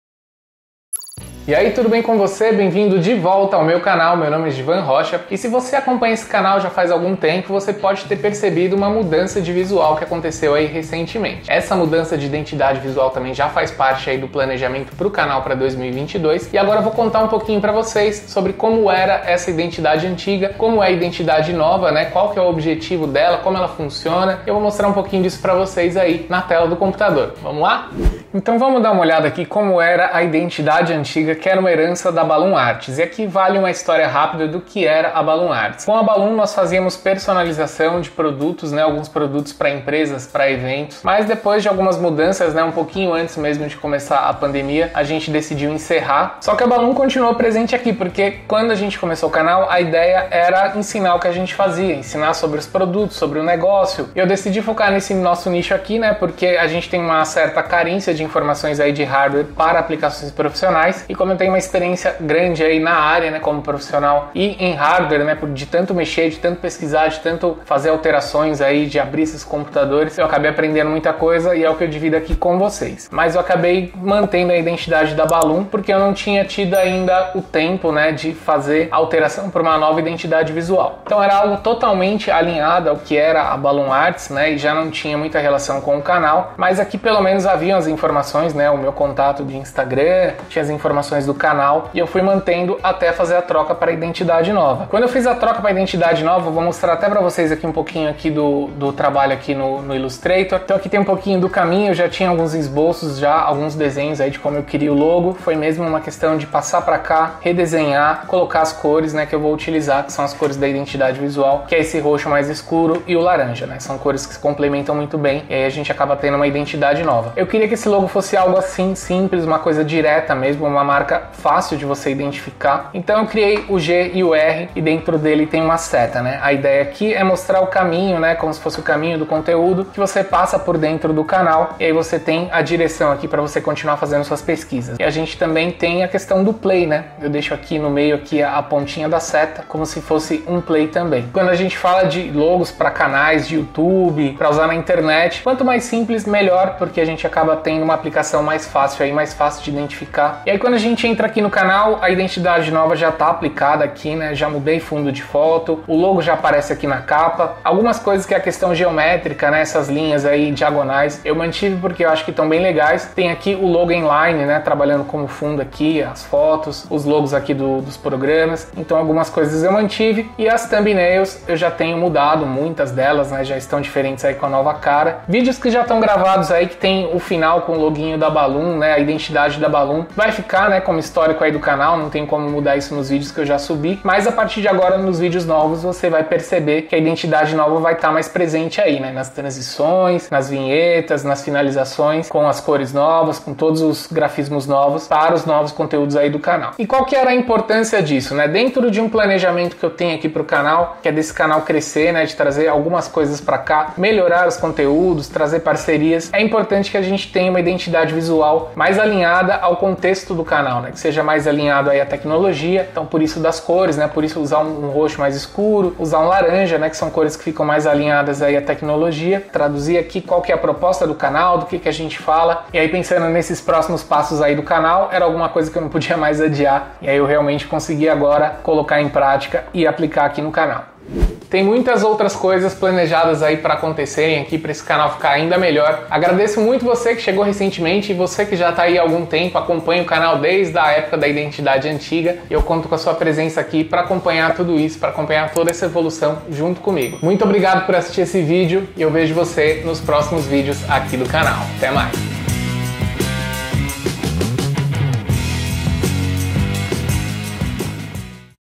🟢 O CANAL ESTÁ DE CARA NOVA | Como criei minha nova identidade visual
Nesse vídeo apresento a nova identidade visual do canal, o novo logo Givan Rocha e conto uma breve história do início da canal e da identidade antiga.

Equipamentos da gravação:
Câmera com lente
Microfone Lapela
Tripé de Câmera

Para ver na sequência:
QUAL O FORMATO CORRETO PARA UM BANNER DO YOUTUBE?